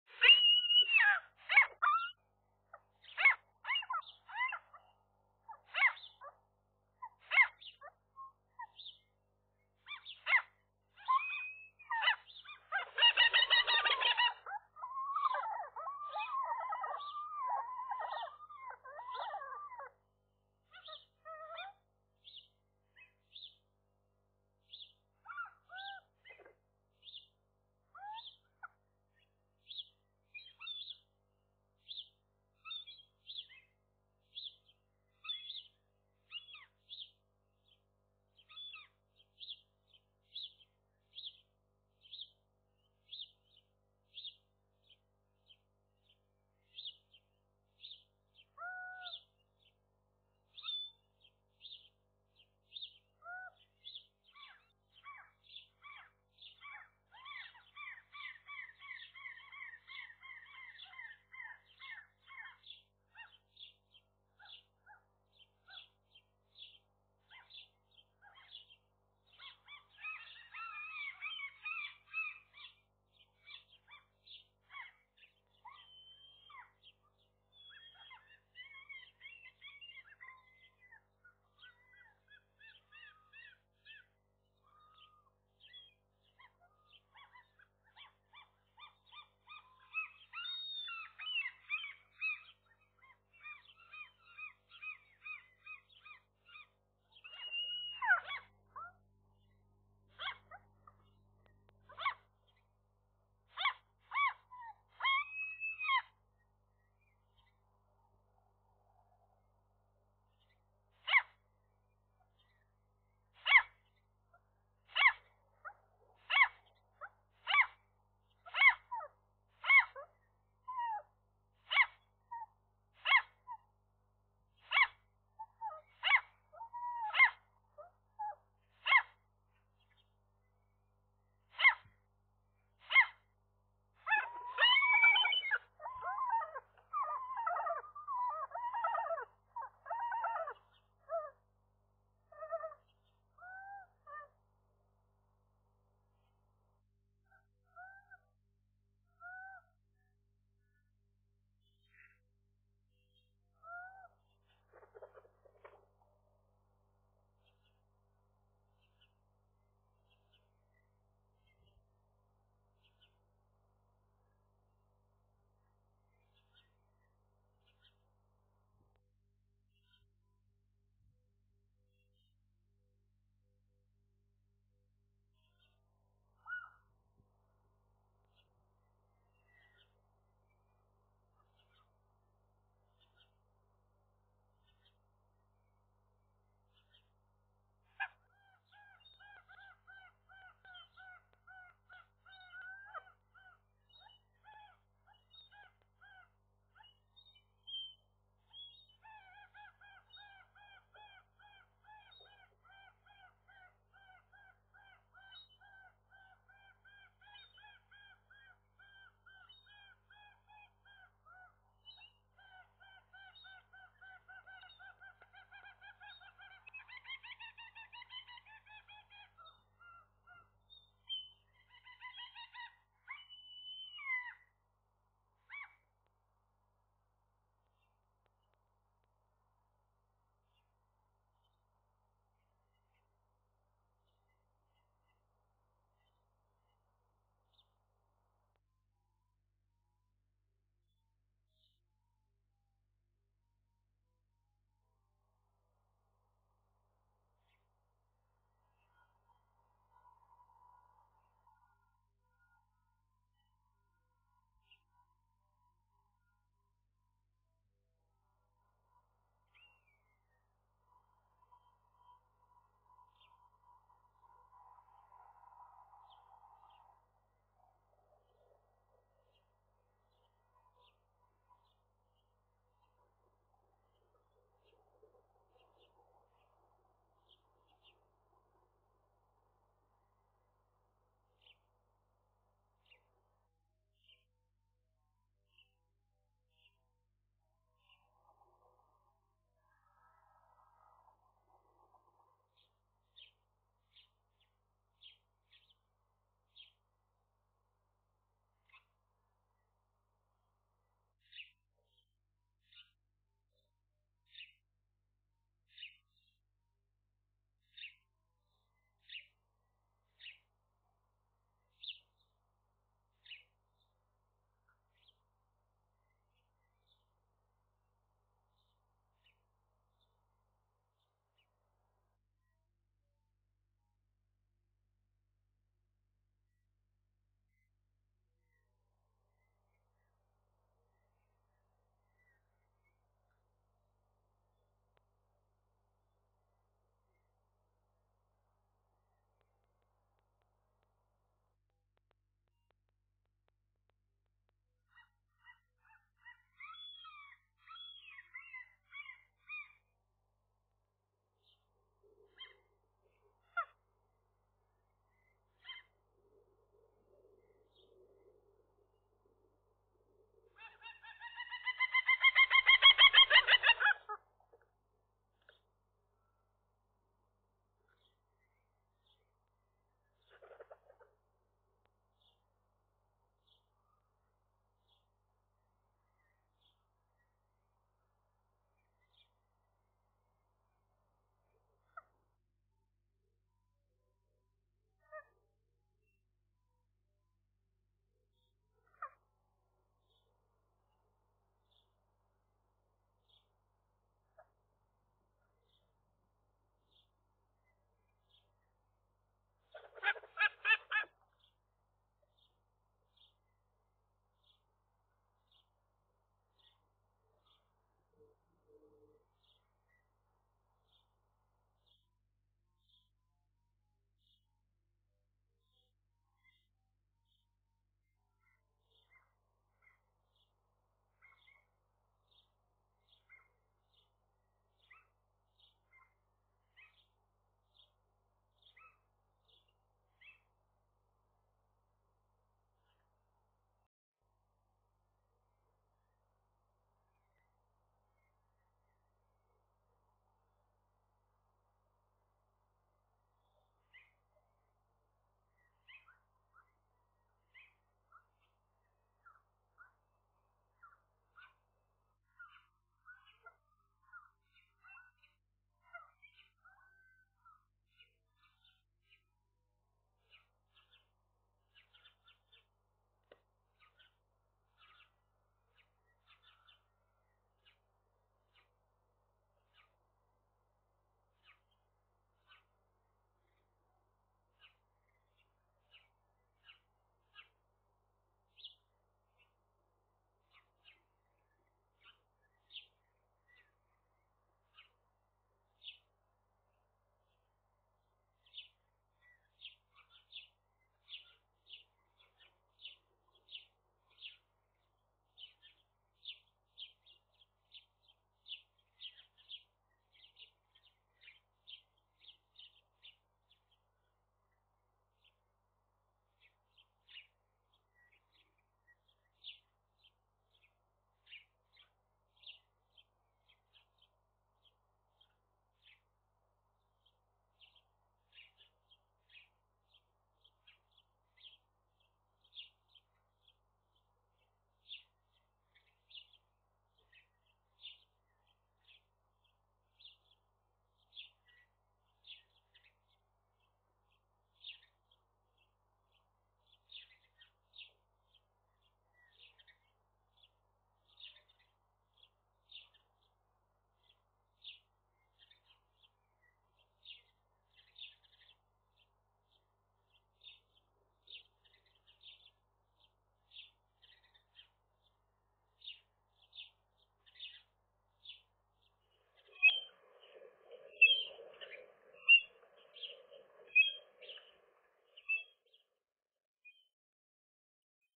230503 seagulls, Gher's and Ghim's 1st egg, ever
Gher laid her first egg this day.
We think the couple is so young they are first timers.
The egg arrived at mid day.
Gher was doing very well (impressive), not very hard laboring.
Ghim stayed close all the time, even comforting her when the egg was about to come.
Gher did the first lying, Ghim after some time.
From now on they will shift on lying.
Most probably there will be three eggs, arriving every second day.